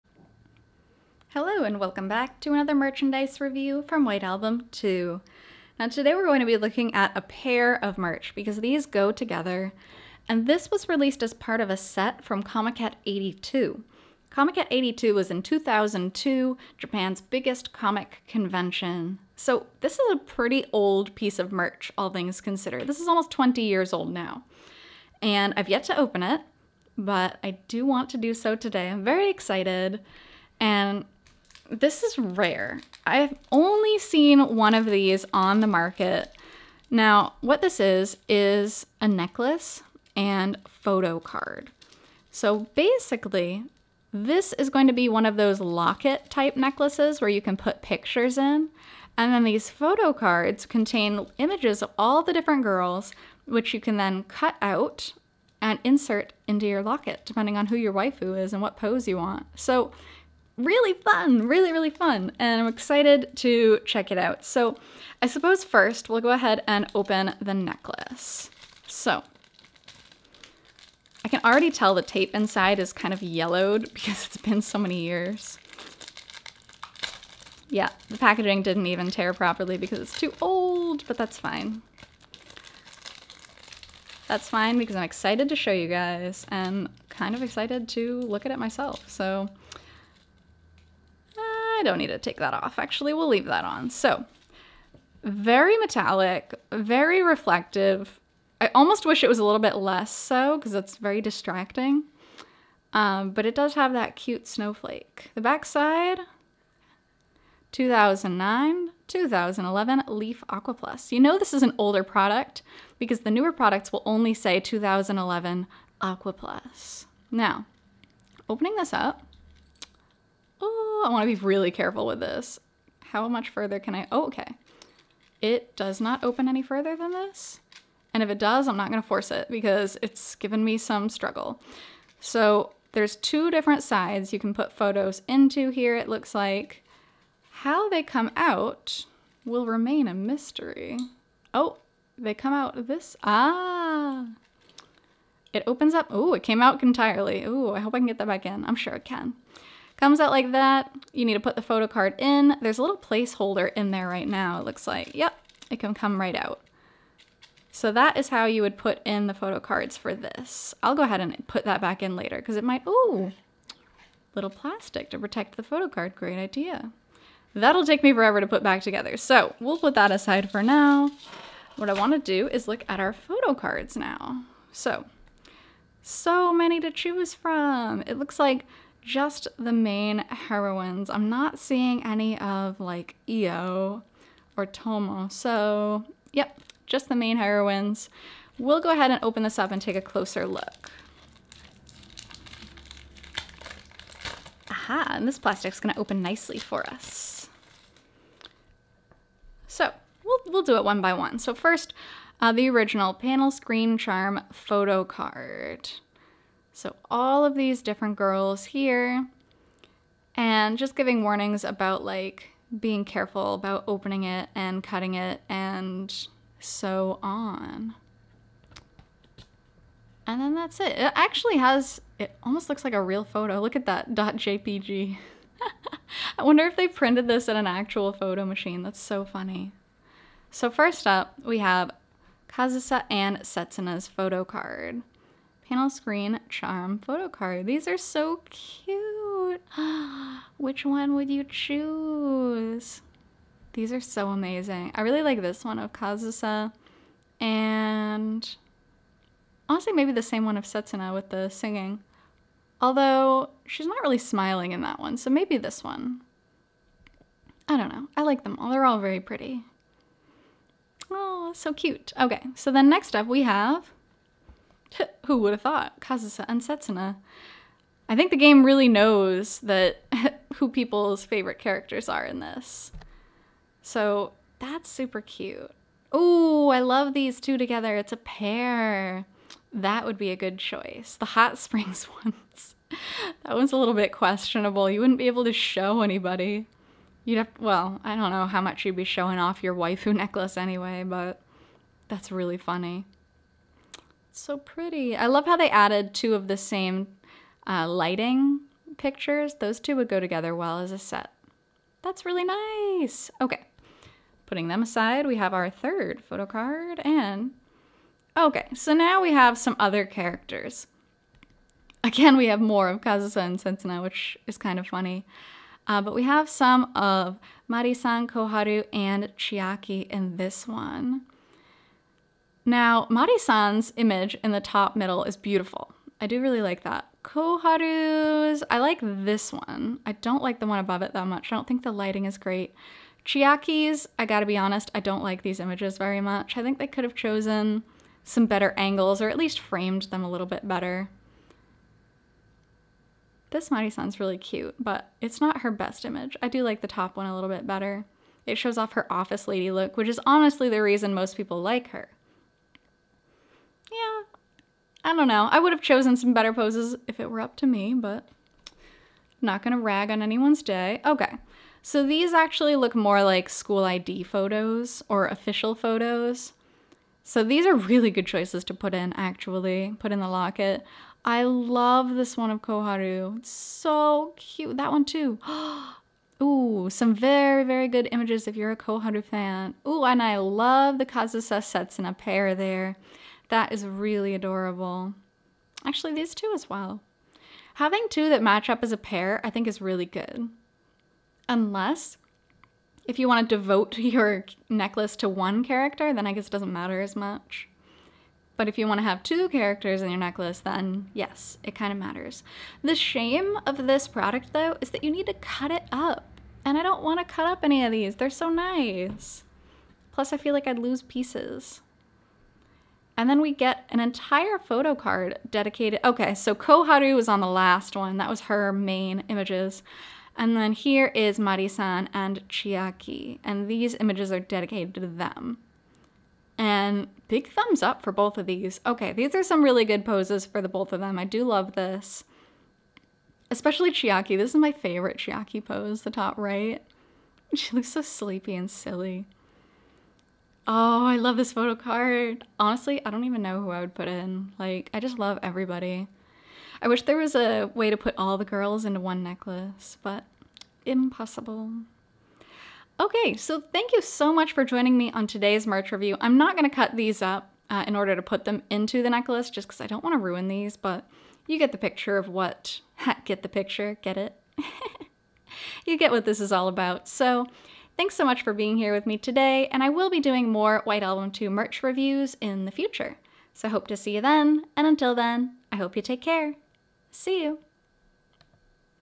Beautiful White Album 2 Photo Card Necklace
Getting to CHOOSE which waifu you want to put in a locket kept close to your heart?? They really wanted to speak to every type of fan with this adorable product~ Which of the 5 heroines would you choose?